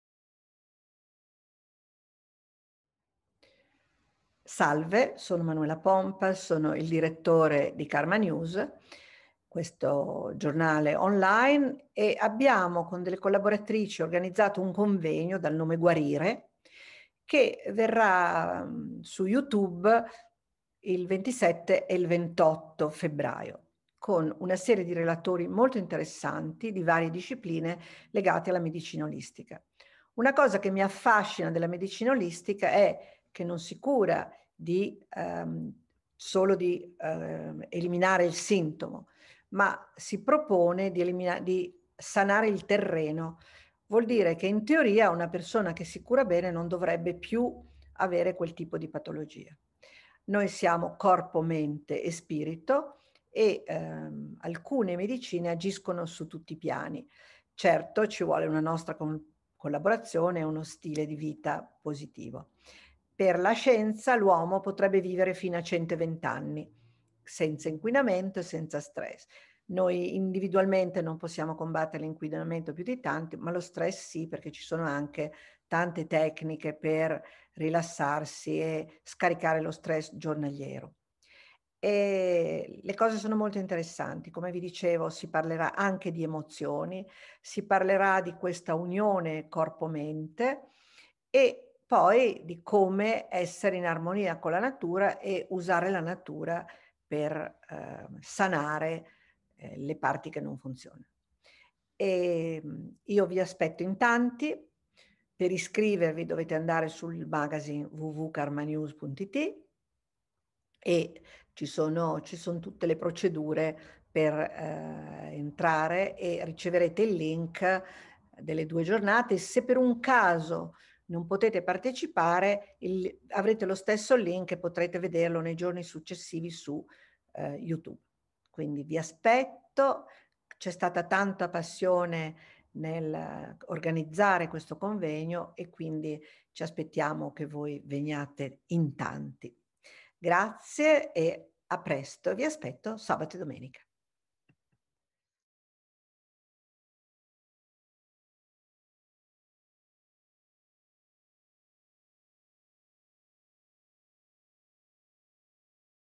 GUARIRE: 27e 28 febbraio, convegno online sulla medicina olistica
Karmanews ha organizzato due giorni di incontri LIVE con relatori molto interessanti, che disquisiranno su varie discipline olistiche.